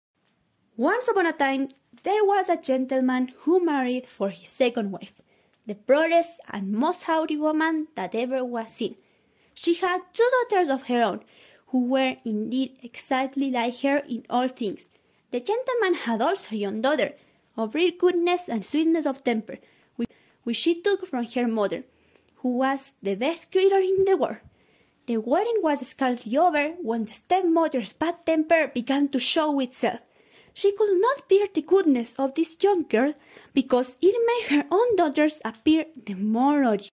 Cinderella - Parte 1
Author Biography.

Charles Perrault, (born January 12, 1628, Paris, France—died May 15/16, 1703, Paris), French poet, prose writer, and storyteller, a leading member of the Académie Française, who played a prominent part in a literary controversy known as the quarrel of the Ancients and Moderns. He is best remembered for his collection of fairy stories for children, Contes de ma mère l’oye (1697; Tales of Mother Goose). He was the brother of the physician and amateur architect Claude Perrault.

Fecha de publicación: 1697 / Editor: S.A. Editorial La Galera, 2007. / No. de páginas: 26 p.

Reader: Paula Valentina López García /8 A- EGBS